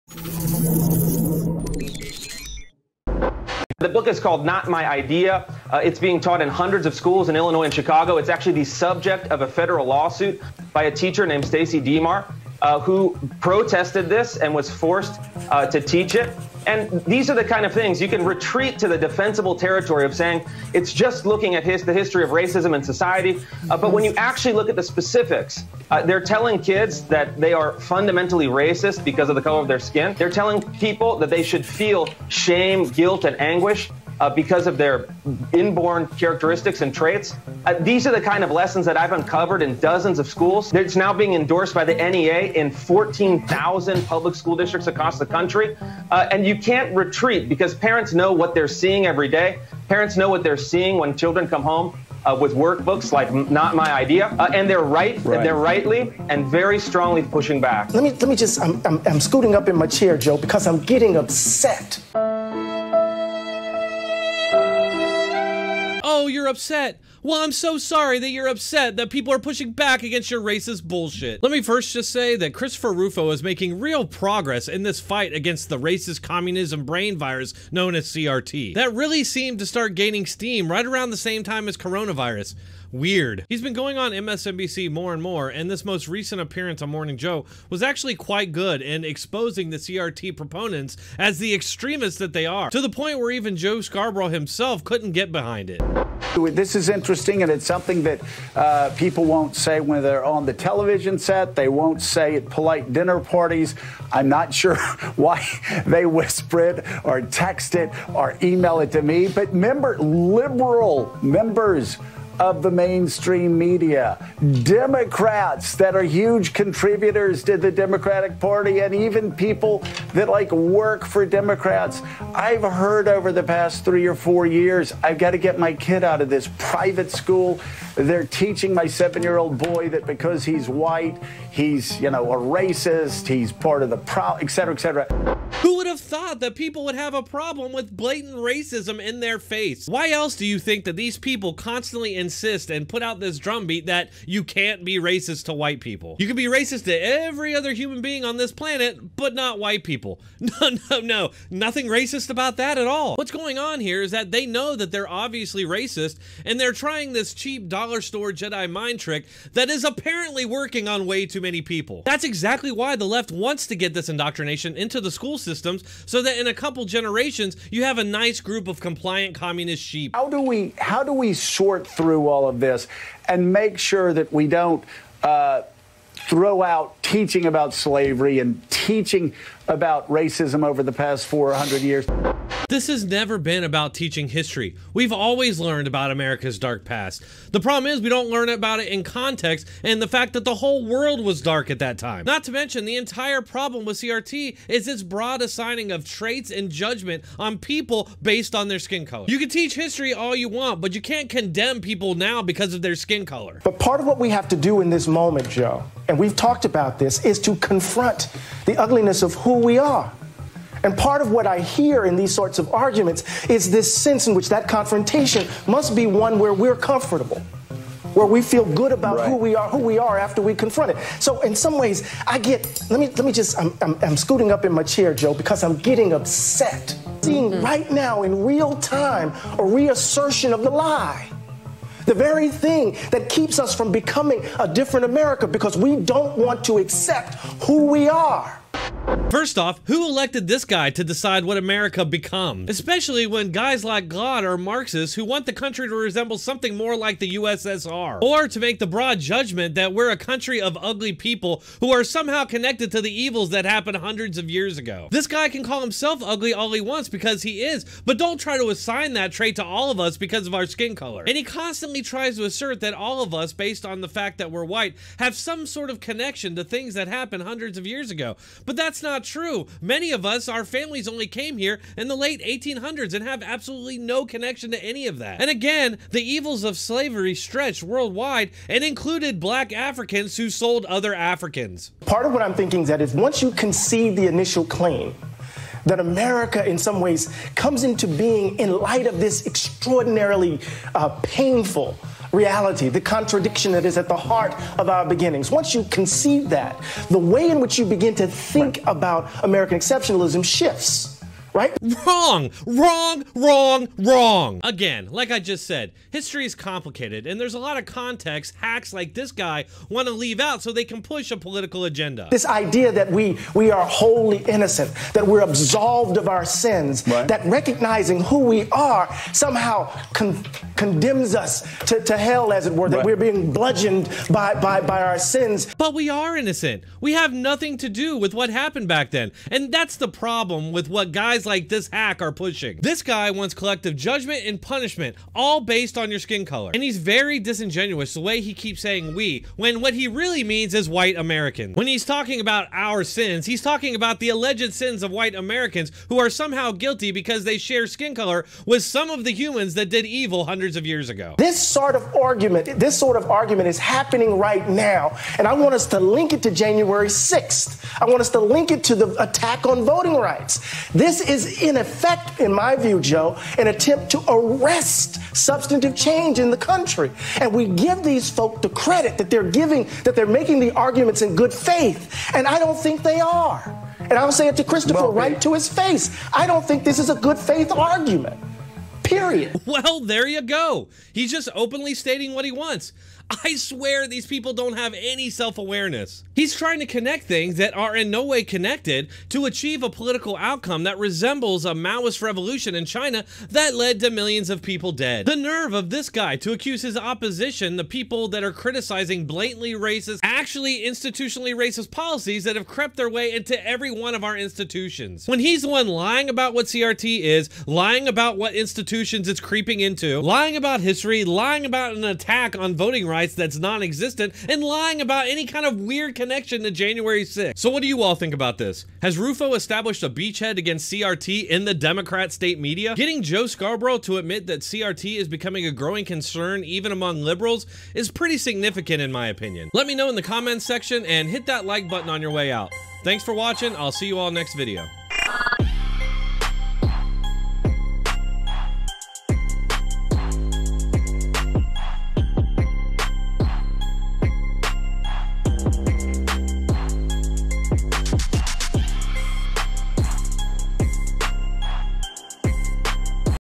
LOL: MSNBC Guest MAD After Getting Schooled on Critical Race Theory
➡️ Sources:

He Can't Handle the Truth! MSNBC's Glaude Melts Down After Being Schooled by CRT Critic